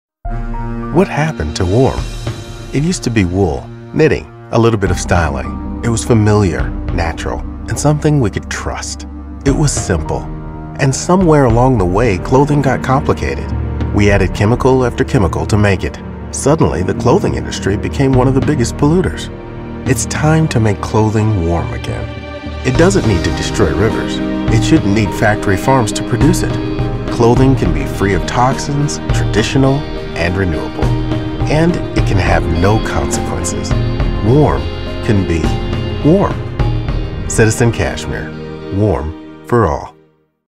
Eco Friendly Clothing | Citizen Cashmere
A look at where staying Warm has been in the past.  Get involved and help us re-think how clothing should be made.